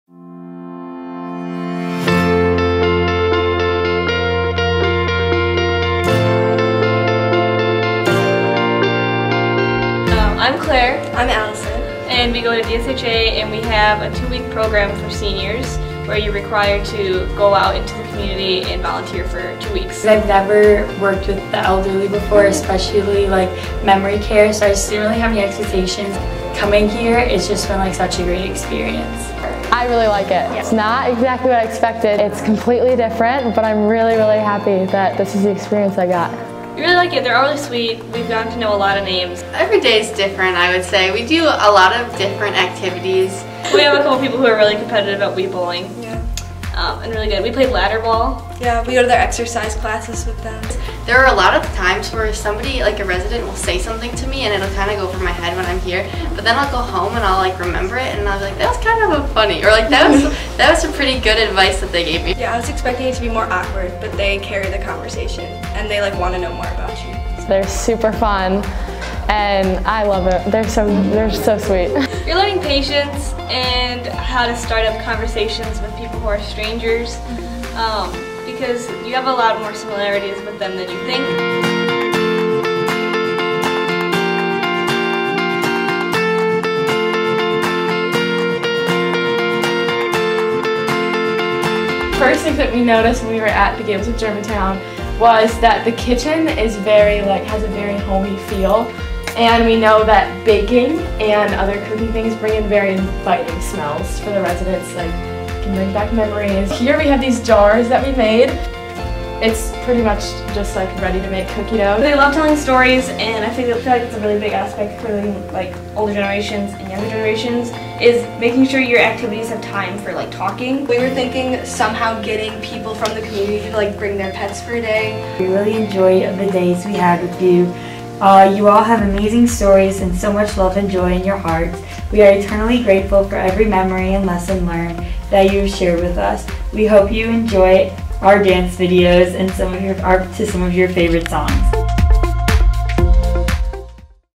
Vocare Experience 2020
April is NationalVolunteerMonth! We are so grateful for all of our volunteers. Today, we are featuring the remarkable Divine Savior Holy Angels High School students who volunteered with us for two weeks through their senior-year Vocare Program. Though their time with our residents was cut short due to COVID-19, we leveraged their creativity and talents to share their ideas on resident engagement through a “Shark Tank” activity to wrap up their experience. They came up with fabulous ideas – check out the video!